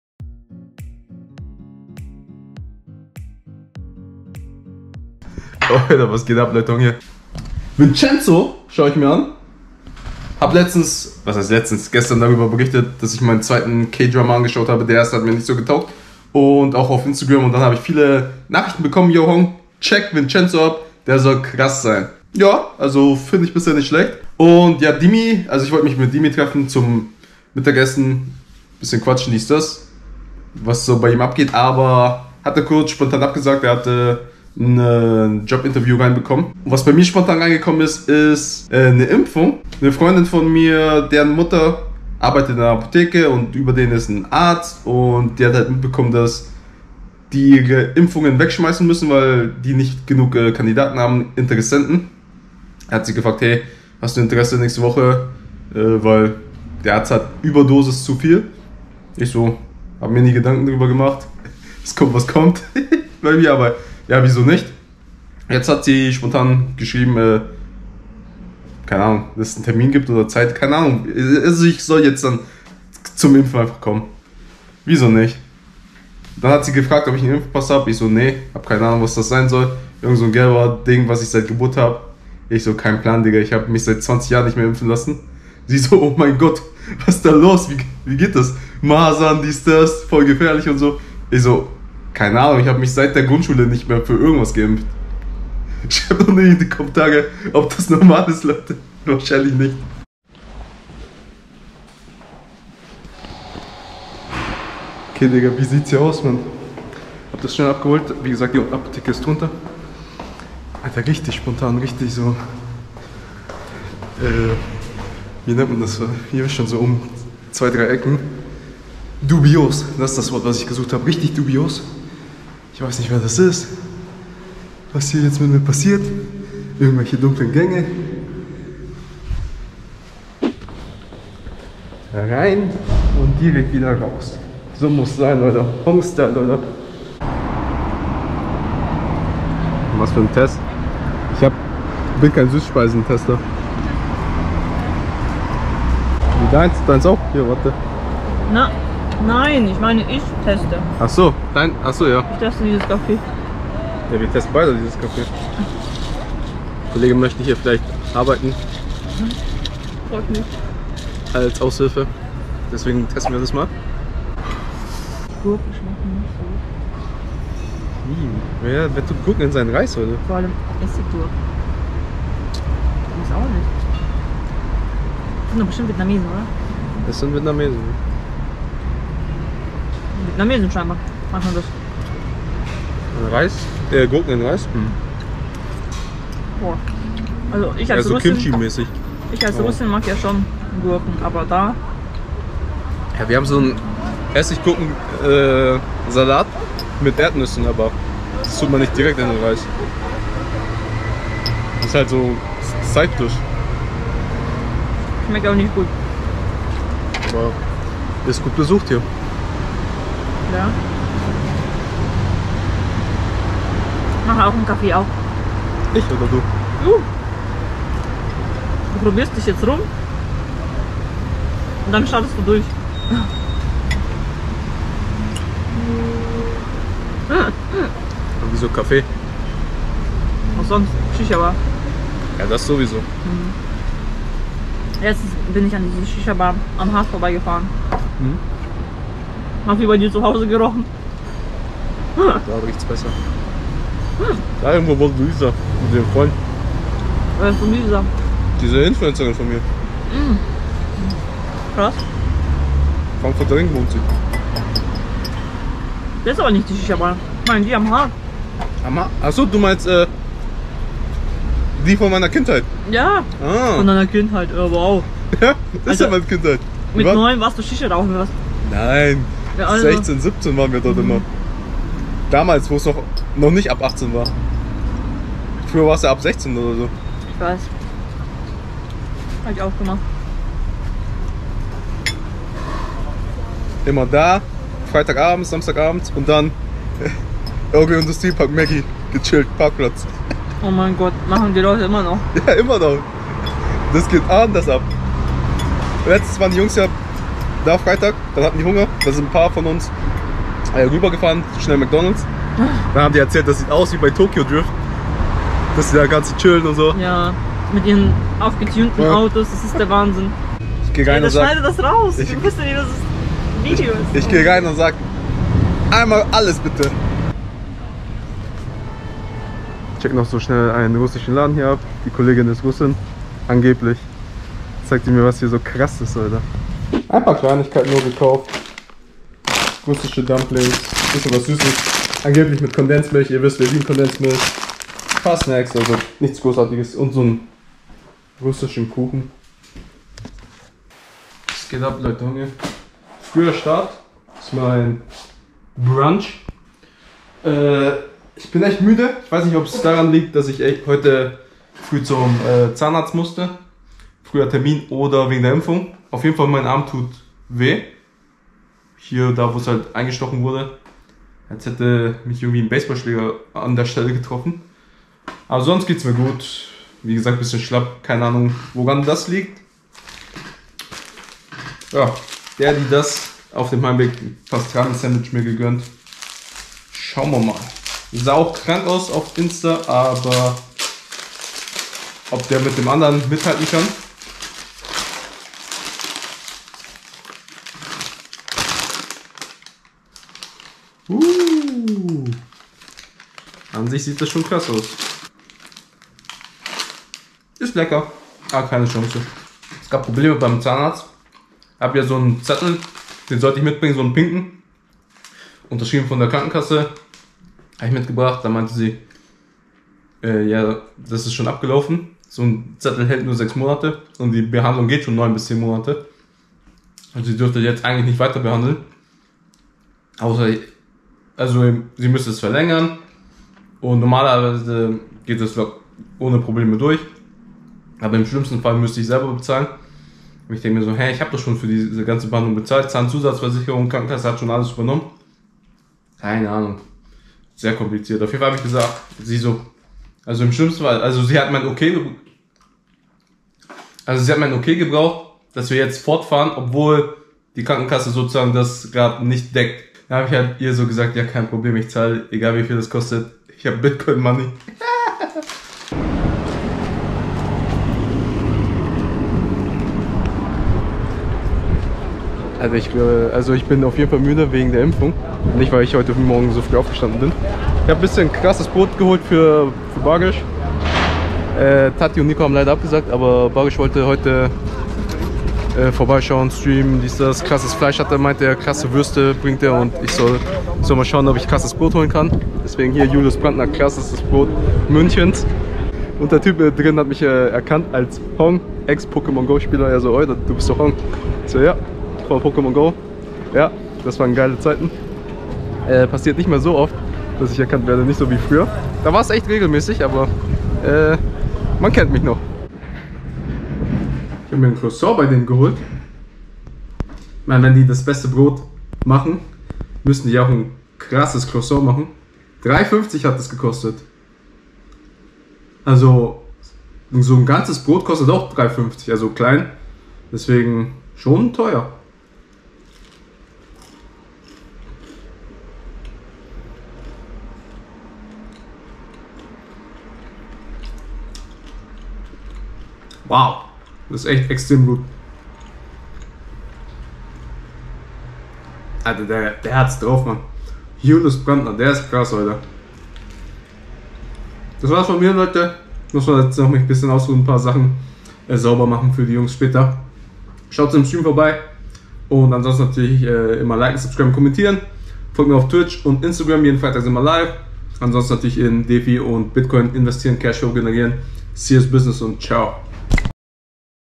SPONTAN GEIMPFT WORDEN?! VLOG 01.07.2021
euda ich habe mich spontan einfach impfen lassen
bin müde...